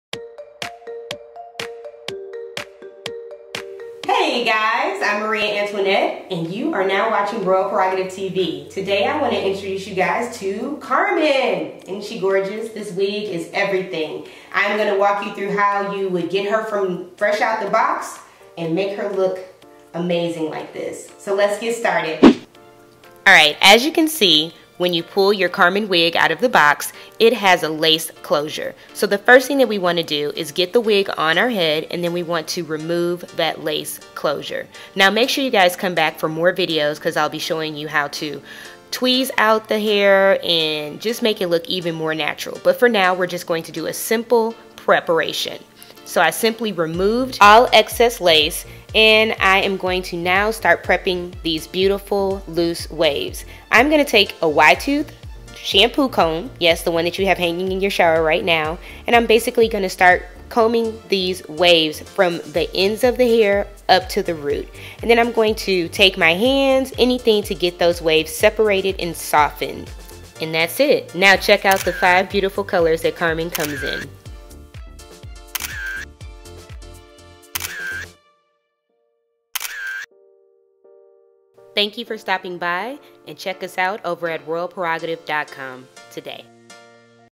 Carmen SynLUXE® Wig | Royal Prerogative TV
Hey RPQueens! I am super excited to introduce you to Carmen SynLUXE® Wig. This heat safe synthetic wig is a total show stopper! See how easy it is to prep her and go! Visit RoyalPrerogative.com to see all of the amazing SynLUXE wigs, curated by Maria Antoinette!

Specs for "Carmen" SynLuxe® Wig:

Type: Lace Closured for Versatile Parting / Top Knots
Cap Construction: Capless
Cap Size: One Size Fits Most
Cap Fit: All wigs come with combs inside to insure a secure fit, as well
as an adjustable straps at the nape
Style: Loose Wave
Material: SynLUXE® Fibers - Heat Safe. Can be styled with hot tools low-medium temperature
Length: 22"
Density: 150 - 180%
Package Contents: 1 x Wig

Instagram.com/RoyalPrerogative
Facebook.com/RoyalPrerogative